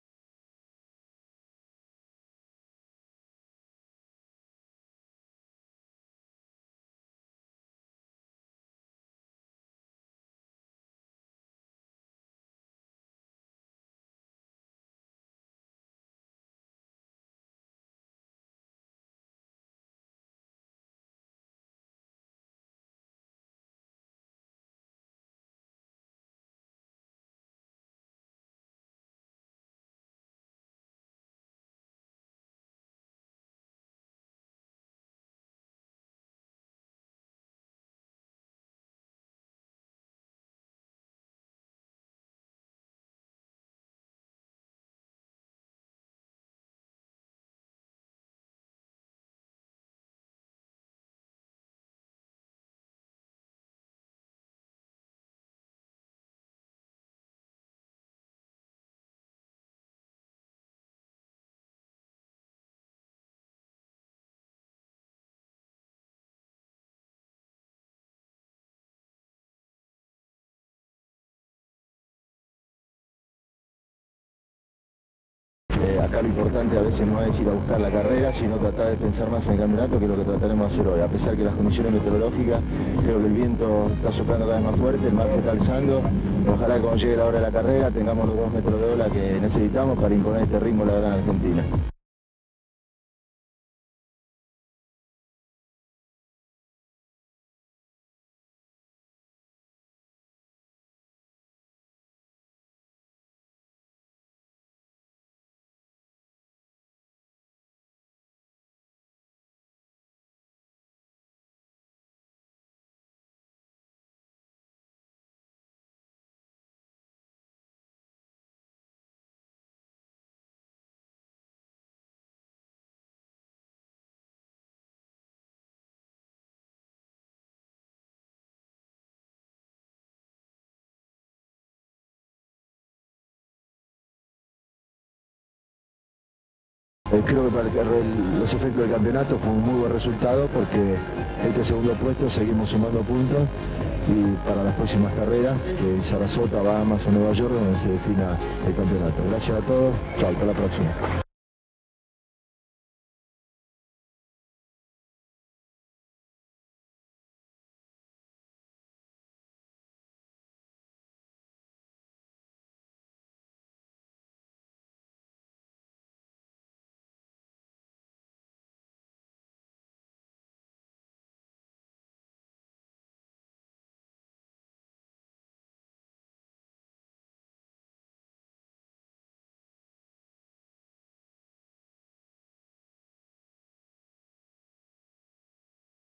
Daniel Scioli - Copa America de Superboat en Miami 1995
Copa America 1995 de Superboat en Miami, Estados Unidos. Compacto de imágenes donde se observa La Gran Argentina (01) de Daniel Scioli. Toma panorámica con edificios en Miami. Imagen de la lancha La Gran Argentina. Imágenes de Daniel Scioli, Juan Alberto Mateyko, Teresa Calandra, Palito Ortega, Karina Rabolini, Teto Medina, Dolores Barreiro, Joaquin Galan, Patricia Miccio y otras figuras argentinas. Motor de la lancha. Fachada de edificios en Miami. Bandera Argentina flameando. Preparativos de las embarcaciones. Testimonio de Daniel Scioli antes de la carrera. Gente en las playas de Miami. Carrera con la lancha La Gran Argentina (01). Helicóptero sobrevolando las embarcaciones. Carrera con las lanchas 10 (Nuff Respect) y 11 (Alcone). Mujer tomando sol recostada en la playa. Carrera con la lancha 63 (Zero Defect). Testimonio de Daniel Scioli sobre la carrera donde consigue el segundo puesto y espera un mejor resultado para la próxima fecha en Sarasota, el 2 de julio. Imagen de Scioli en el podio recibiendo el trofeo.
Fecha: 11/6/1995
Duración: 2 minutos 50 segundos
DV-21830